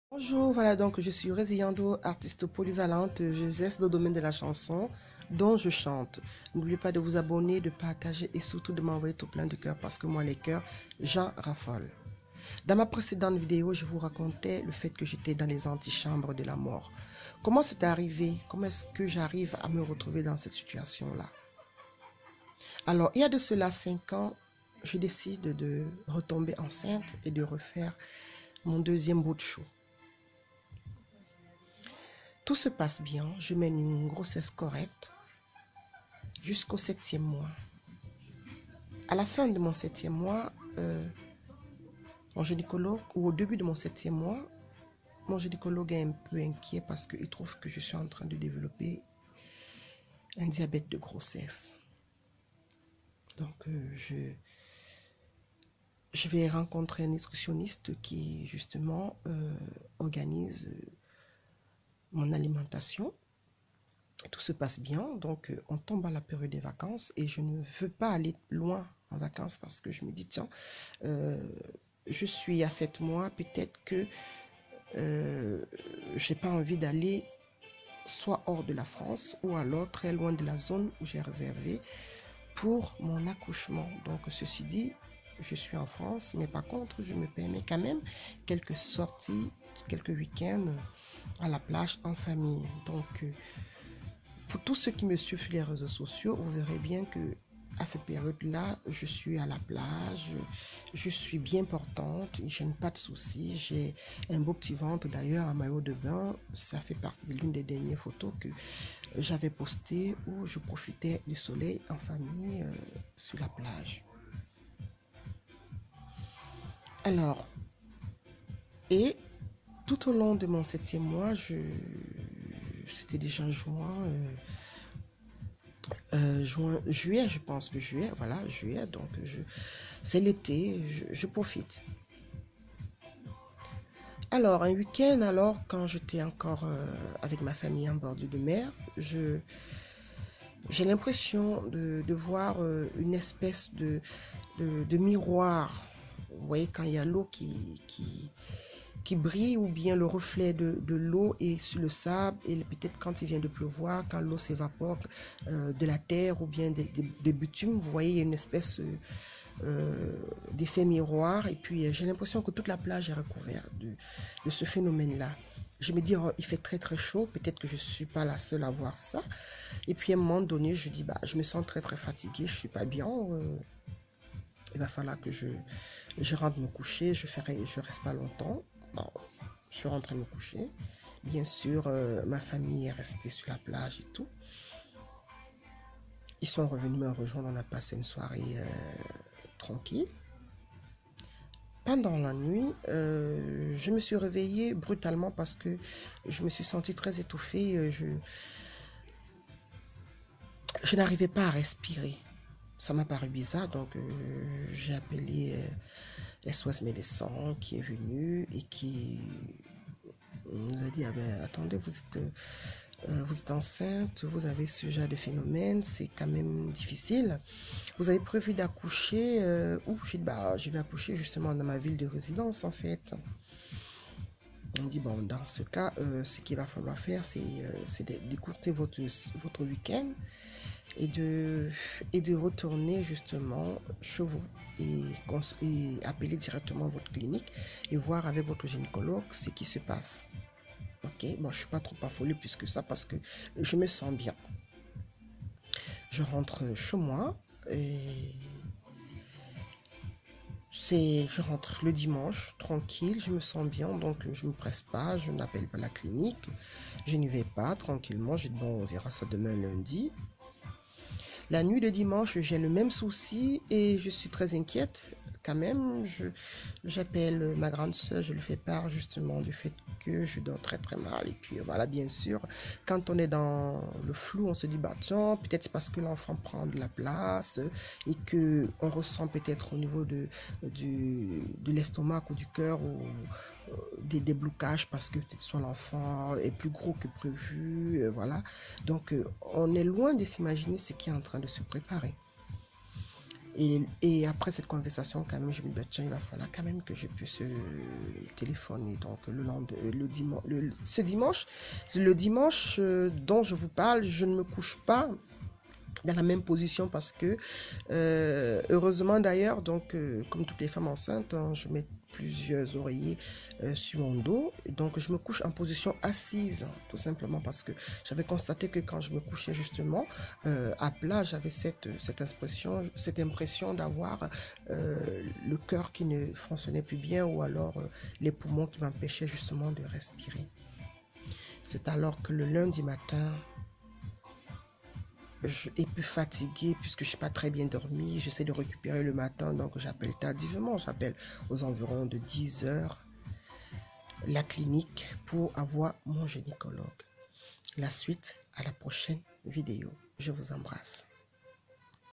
DU 1er au 7éme MOIS DE MA GROSSESSE, RIEN NE PRESAGEAIT UNE GROSSESSE COMPLIQUÉE
DU 1er au 7éme MOIS DE MA GROSSESSE, RIEN NE PRESAGEAIT UNE GROSSESSE COMPLIQUÉE  PENDANT LAQUELLE  J'AI FROLÉ LA MORT IL Y'A 4 ANS.  SÉRIE Nº2 DES VIDÉOS  OÙ JE VOUS RACONTE MON HISTOIRE DANS LE BUT DE PRÉVENIR TOUTES LES FEMMES DU MONDE QUI VIVENT OU QUI ONT VÉCU LA MÊME CHOSE, OU QUI SONT SUCEPTIBLES DE DÉVELOPPER CETTE MALADIE DE GROSSESSE TRÈS GRAVE QUI  EST LA 2ēme CAUSE DE DÉCÈS MATERNEL EN FRANCE,  LA 3ēme CAUSE DE DÉCÈS DANS LE MONDE ET 30% DE DÉCÈS MATERNELS 20% DE DÉCÈS FŒTAUX EN AFRIQUE ET POURTANT CETTE MALADIE EST MAL CONNUE PAR LE GRAND PUBLIC